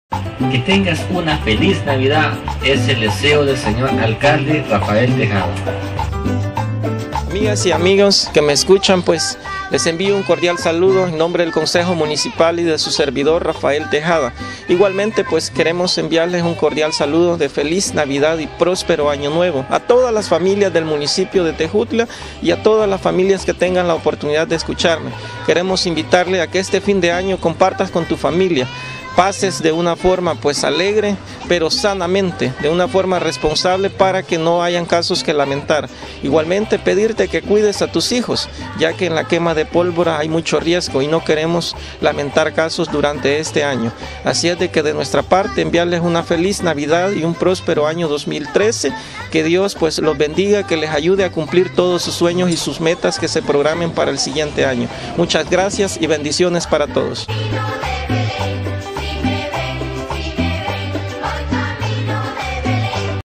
Saludo Navideño Alcalde Rafael Tejada Tejutla Chalatenango
Un Saludo Navideño de El Señor Alcalde de Tejutla